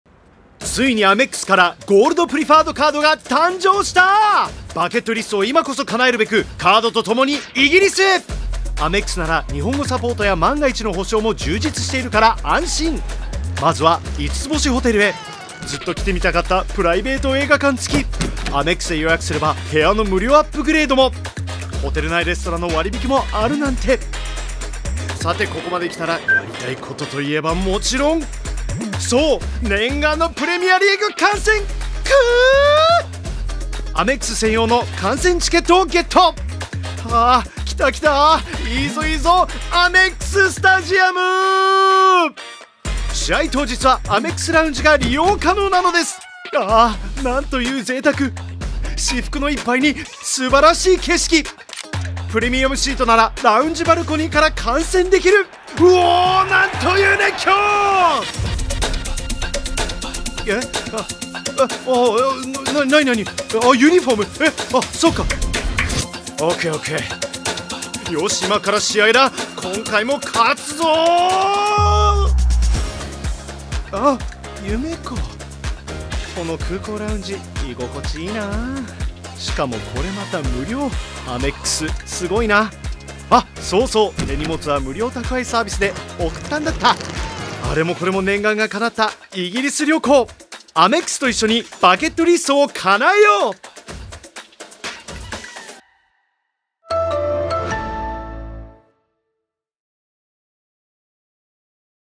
【イギリス一人旅】アメックスで叶える3つのバケットリスト
アメリカン・エキスプレスから新登場した「ゴールド・プリファード・カード」を使って、夢のイギリス旅行へ。ロンドンの人気スポットや、プライベート映画館付きのホテル、そして本場プレミアリーグの生観戦も。詳しくはFASHIONSNAP記事をチェック！

夢のイギリス一人旅　「アメックス」の新ゴールド・プリファード・カードで叶える3つのバケットリスト(PR)︎

Director/DP: Shunsuke Nakamura
Editor/Camera Operator: Akira Polenghi
Producer: Tom Harwood
Grip: Soki Nojirino
Colorist: Haimei
Colorist Assistant: Color Boy
Sound Operator: Gonçalo BC
Sound Engineer: Masaya Tamura
Narrator: Matthew Masaru Barron